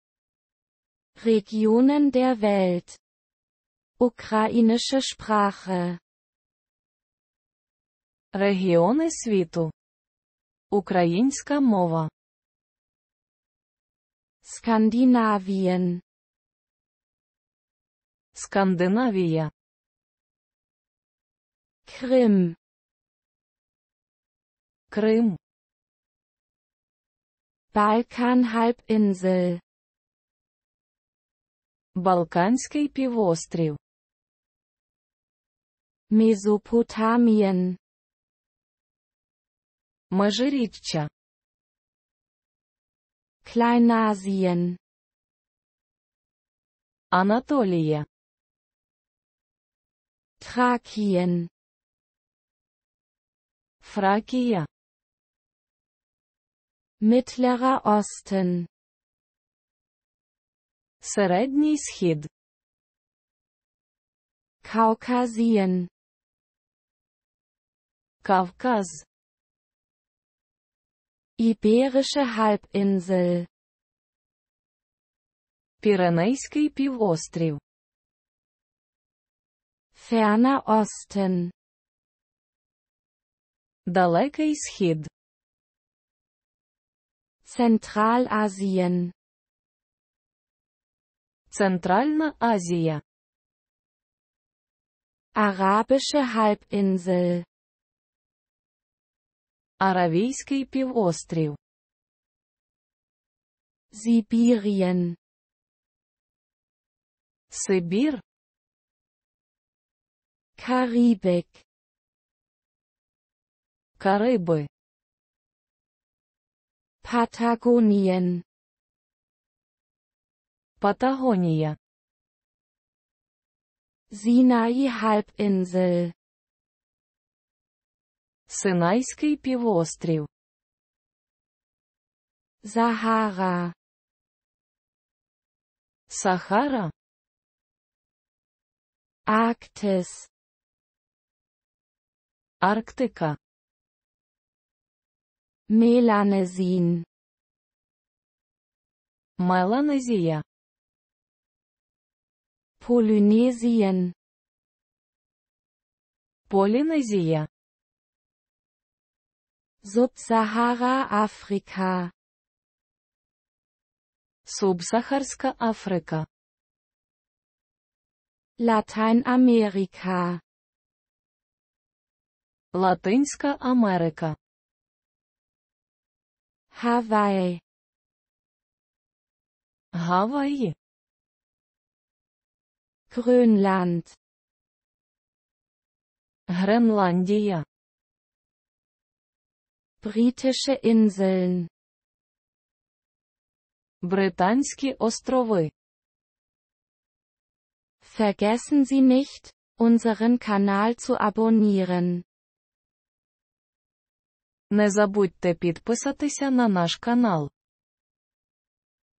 Regionen der Welt (Ukrainische Sprache) (de-uk)
Fone TV
de-uk-33
Регіони світу (Українська мова)
Skandinavien - Скандинавія
Krim - Крим
Balkanhalbinsel - Балканський півострів
Mesopotamien - Межиріччя
Kleinasien - Анатолія
Thrakien - Фракія
Mittlerer Osten - Середній Схід
Kaukasien - Кавказ
Iberische Halbinsel - Піренейський півострів
Ferner Osten - Далекий Схід
Zentralasien - Центральна Азія
Arabische Halbinsel - Аравійський півострів
Sibirien - Сибір
Karibik - Кариби
Patagonien - Патагонія
Sinai-Halbinsel - Синайський півострів
Sahara - Сахара
Arktis - Арктика
Melanesien - Меланезія
Polynesien - Полінезія
Subsahara-Afrika - Субсахарська Африка
Lateinamerika - Латинська Америка
Hawaii - Гаваї
Grönland - Гренландія
Britische Inseln - Британські острови